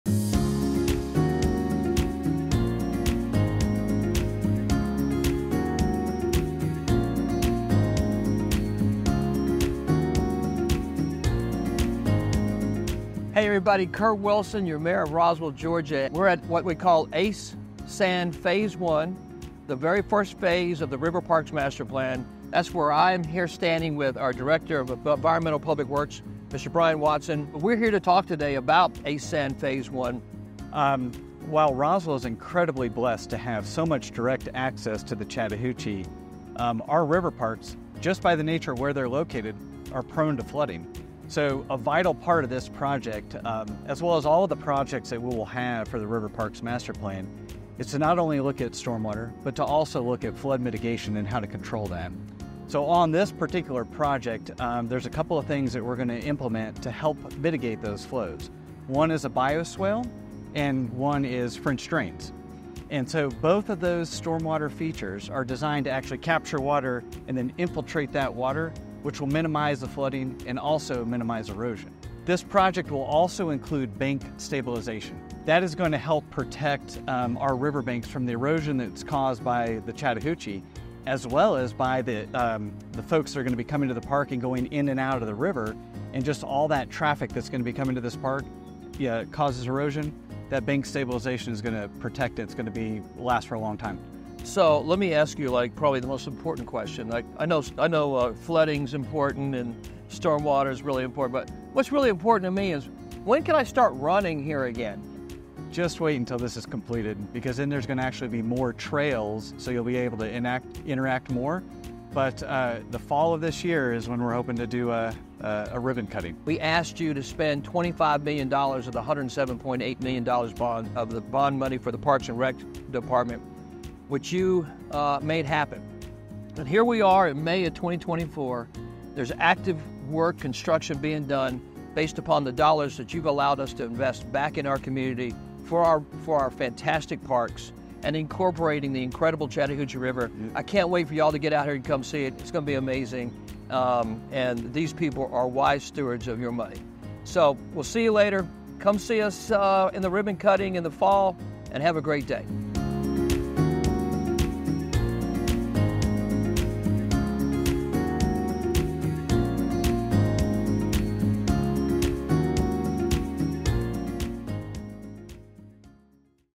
Roswell’s River Parks: Ace Sand Phase I Stormwater and Flood Control Projects
Mayor Kurt Wilson is joined by Brian Watson, Director of Environmental/Public Works, at the site of the Ace Sand Phase I project on Riverside Road. This is the first project of the River Parks Master Plan, which will redefine Roswell’s riverscape.

When Ace Sand Phase I is complete—in fall 2024—the park will feature a multi-use trail, boardwalk, pedestrian trails, and a series of adult fitness facilities. But it will also incorporate creative and innovative features to manage storm and flood waters and to stabilize the riverbank.